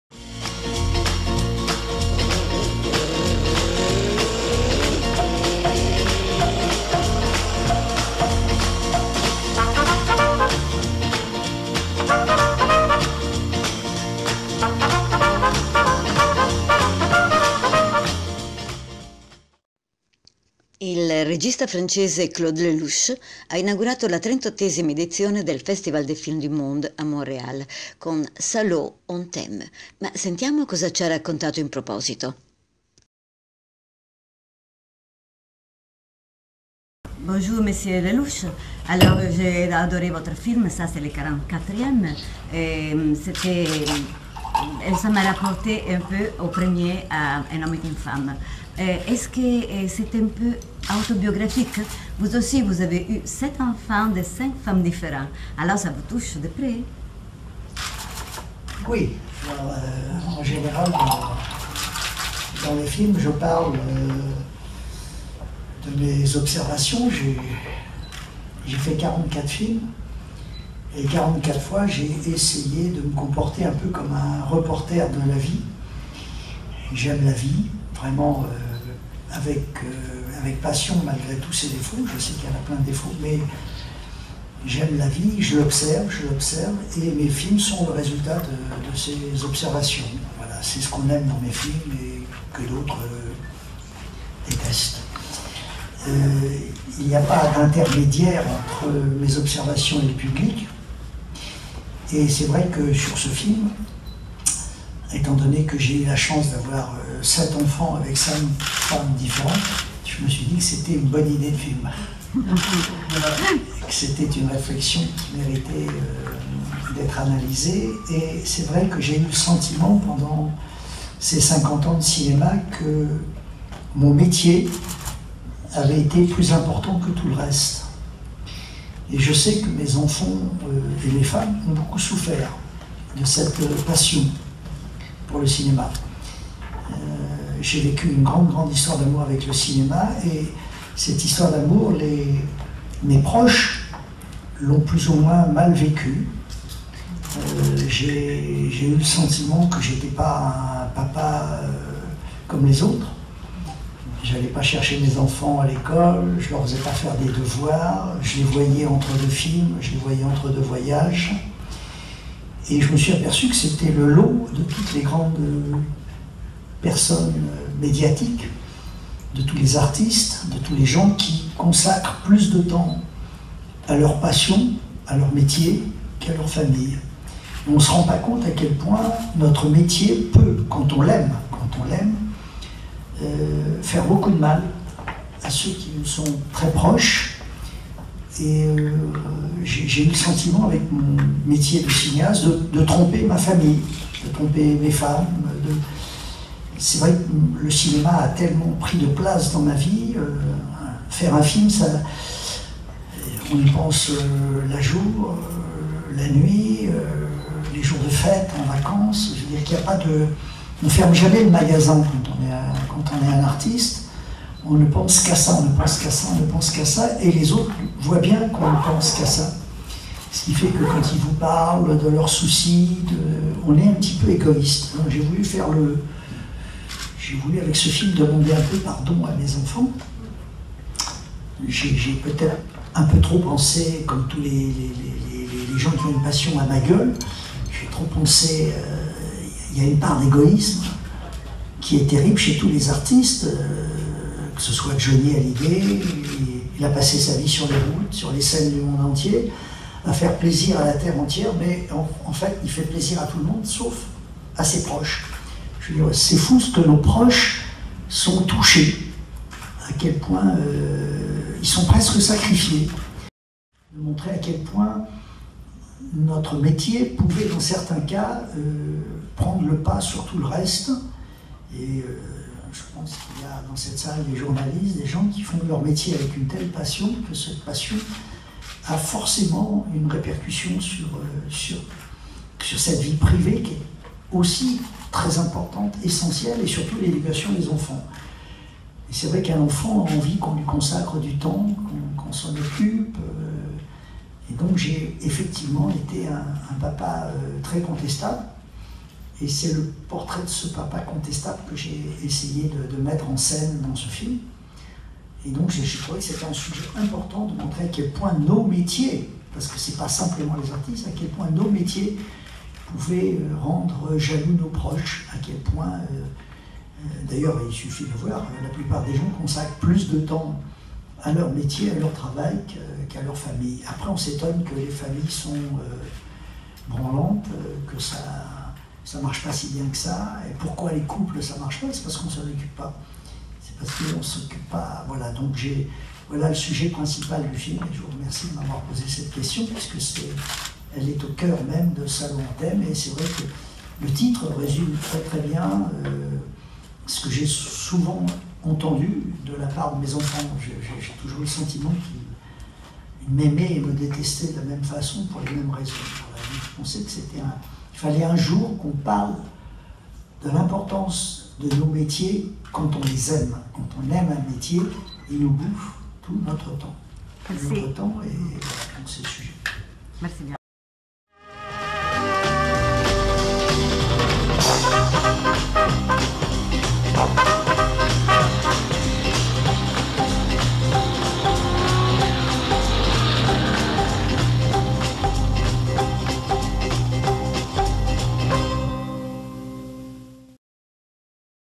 "Salaud, on t'aime" di Claude Lelouch apre il Festival des Films du Monde de Montréal 2014
- Il regista francese Claude Lelouch ha inaugurato la 38^ edizione del Festival des Films du Monde a Montréal con il film “Salaud, on t'aime” (We Love You, You Bastard) interpretato da Johnny Hallyday, Eddy Mitchell e Sandrine Bonnaire.  Ascoltiamo cosa ha raccontato in proposito nel servizio realizzato da Mariangiola Castrovilli.